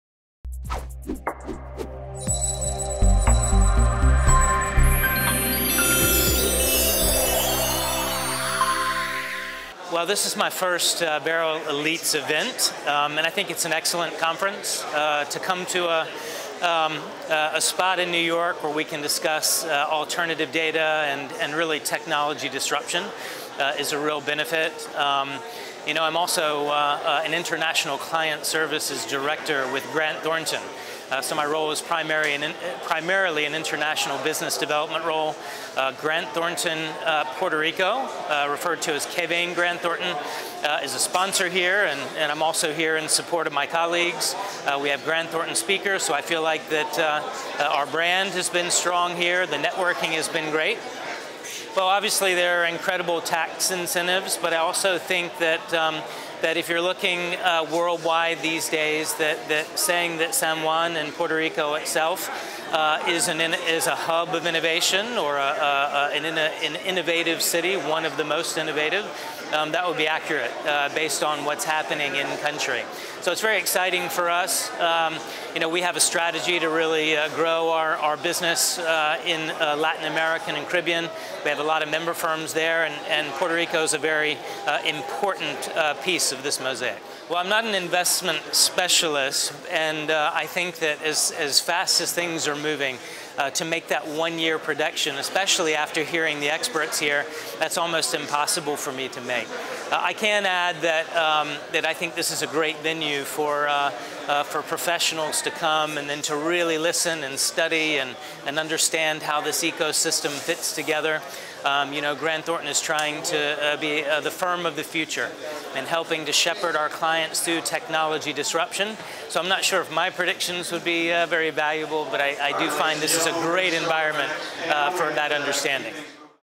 Jason Levergood of Grant Thornton discusses technology disruption in the investment management
BERYL KNOWLEDGE CENTER

Jason Levergood, International Client Services Director on the US National Advisory Services team of Grant Thornton discusses modern Puerto Rico and shares his observations and impressions about Beryl Elites conference.